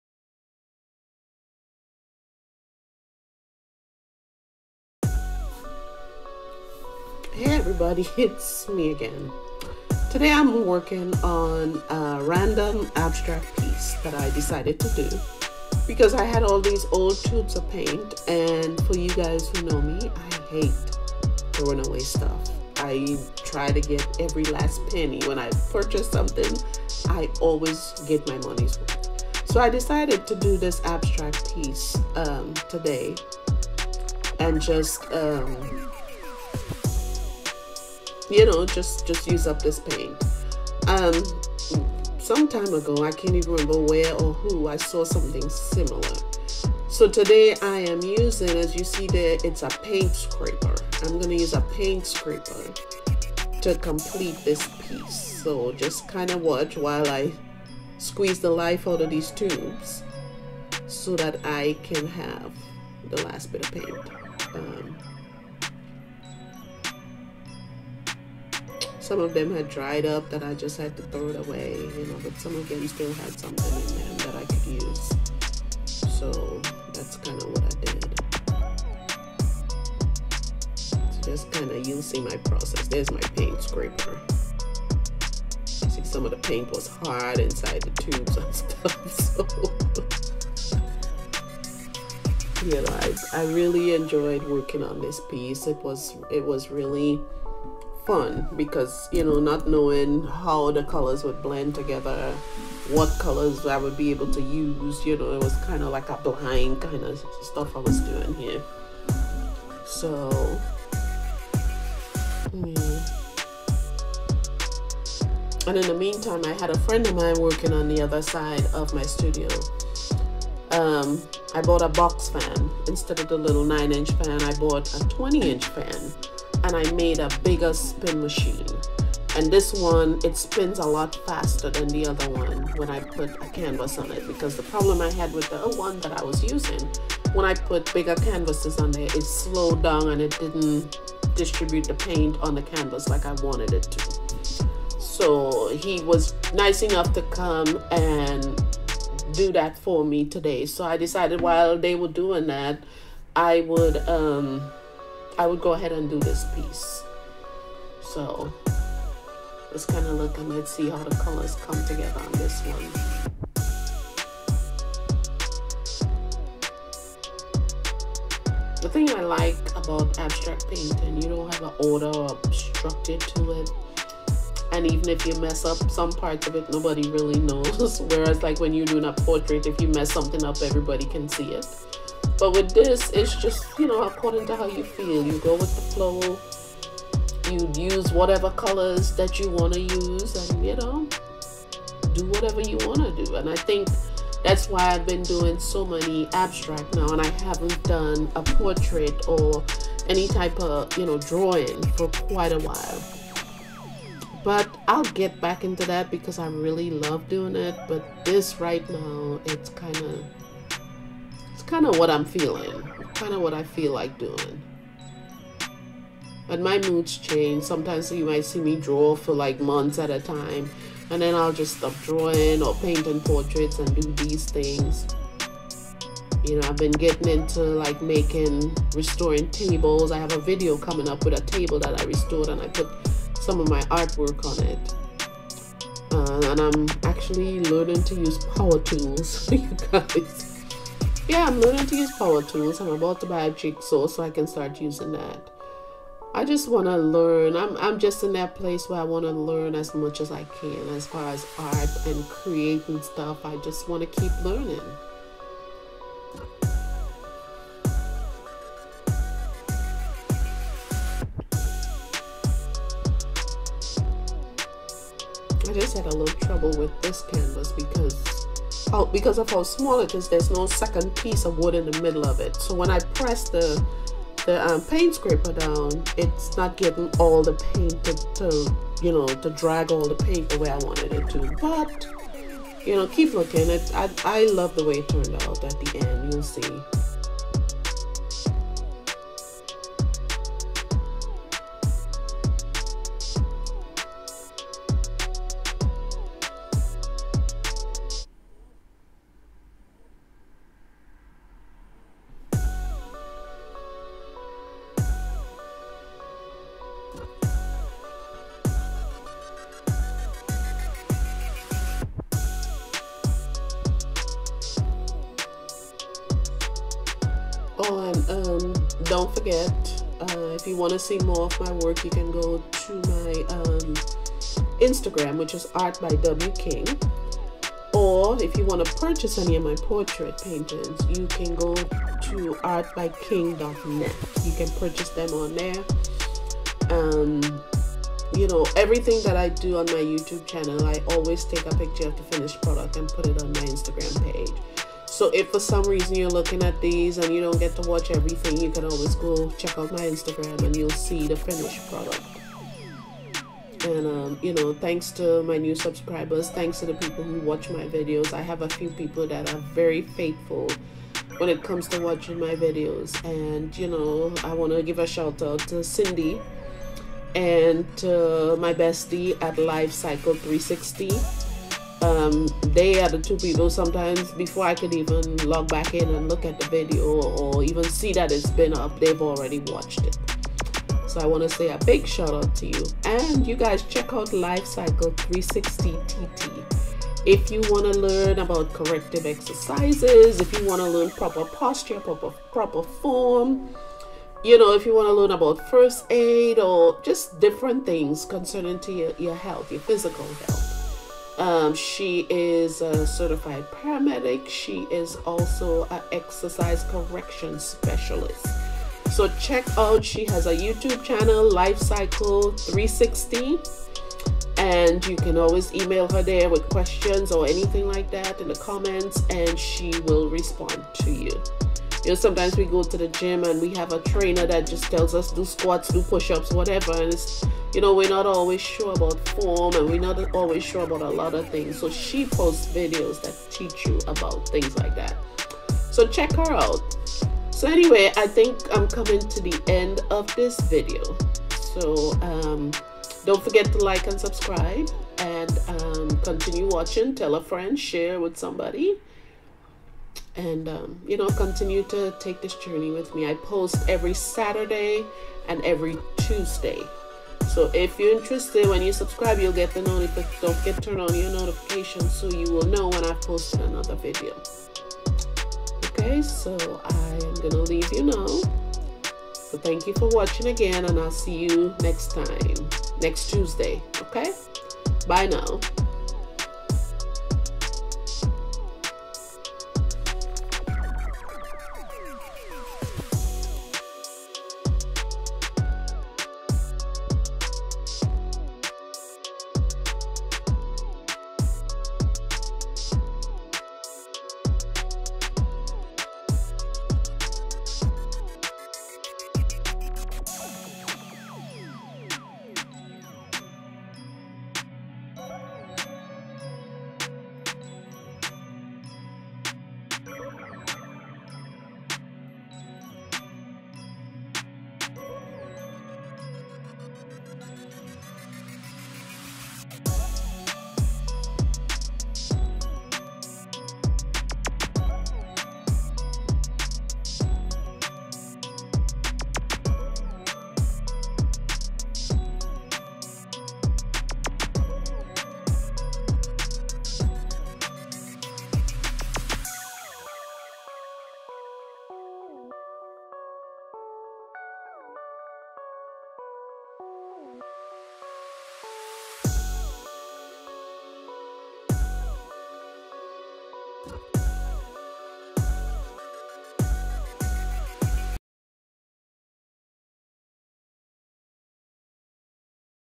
Scrape art for beginners.
I discovered scrape art recently and decided to try it out for myself.  As a beginner I am really excited about this technique so I hope you enjoy this video.